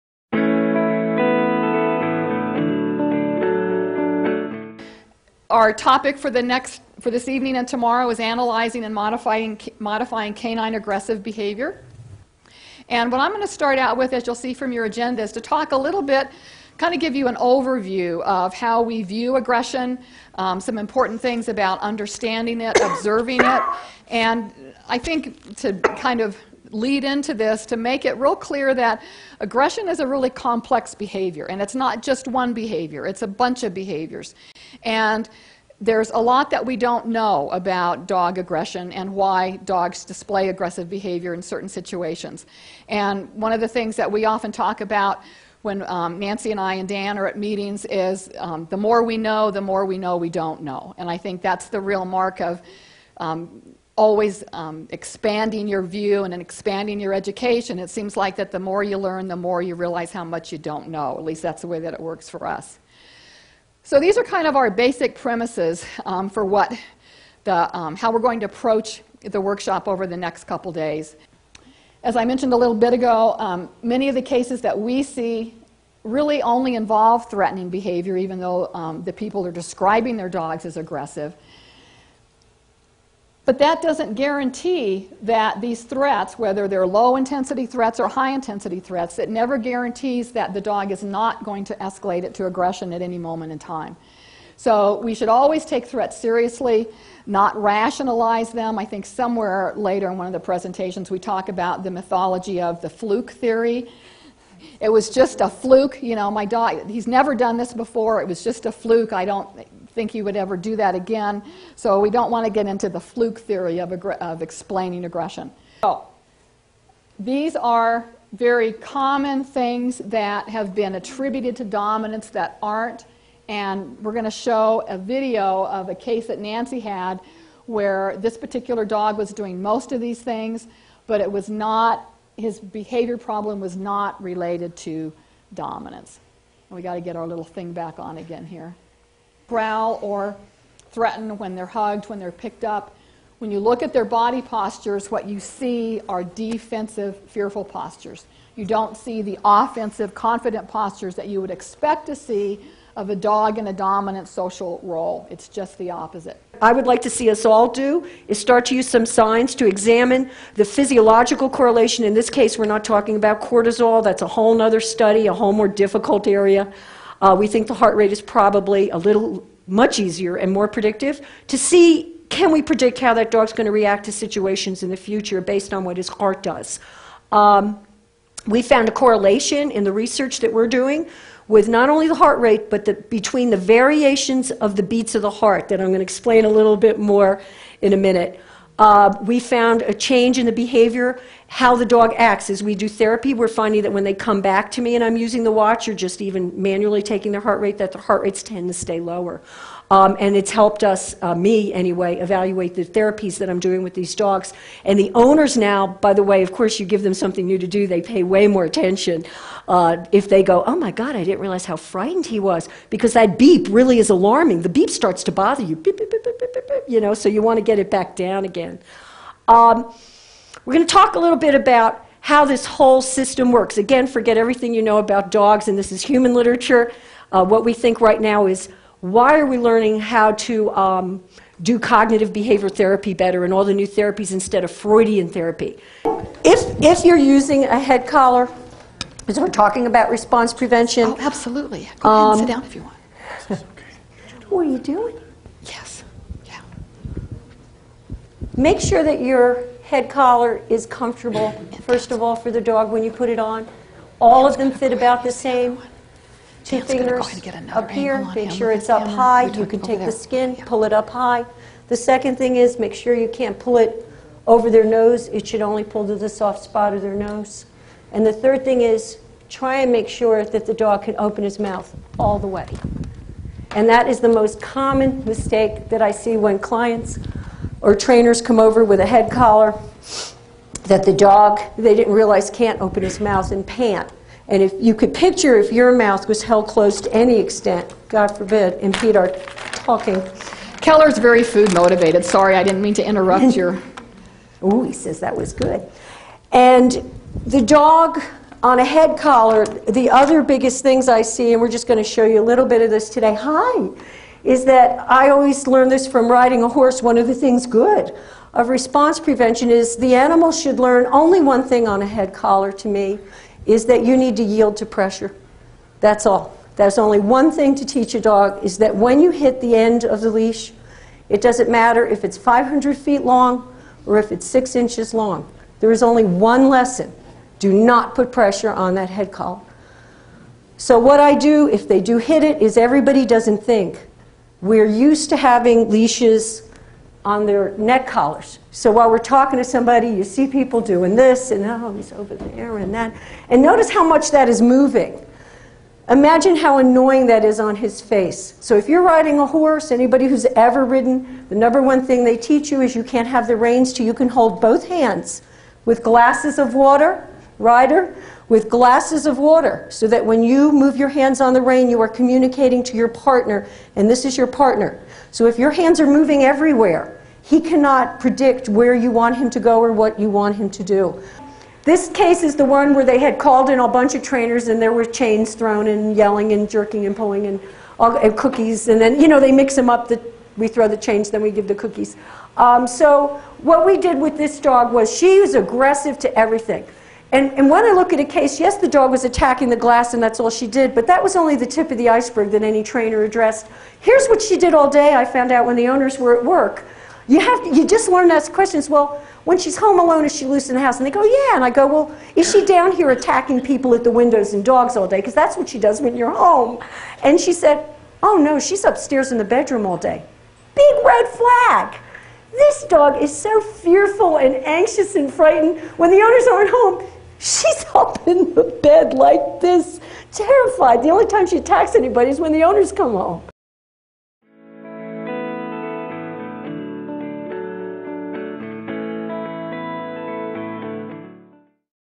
Analyzing and Modifying Aggression - Suzzane Hetts & Nancy Williams
Analyzing & Modifying Canine Aggressive Behavior

Presented by: Suzanne Hetts and Nancy Williams

This Video qualifies for 4 trainer & 5 behavior consultant CCPDT CEUs

This workshop takes an ethological approach to the complexity of canine aggression, discussing agonistic behaviors and the various factors – both internal and external – that influence them. “Mythological” interpretations of social hierarchies and the meaning of social dominance are discussed, and how popular use of these ideas is divorced from the scientific literature. Using case examples and material from veterinary behaviorists, examples of medical problems mis-interpreted as behavior problems are presented.

Risk assessment of dogs including how an evaluation is conducted, the conclusions drawn from it, and the type of report or recommendations made, is discussed from the perspective of individual owners as well as animal shelters and breed rescue groups. Evaluations for legal purposes are touched on briefly.

Ms. Williams describes and demonstrates her work monitoring heart-rate in dogs during behavior modification and implications this may have for behavior assessments and welfare.

An overview of operant and classical conditioning as well as other behavior modification techniques are explained including operant and classical counter conditioning, response prevention, desensitization and when the selective use of positive punishment can be appropriate. The workshop closes with case examples using videos and interactive work with several dogs.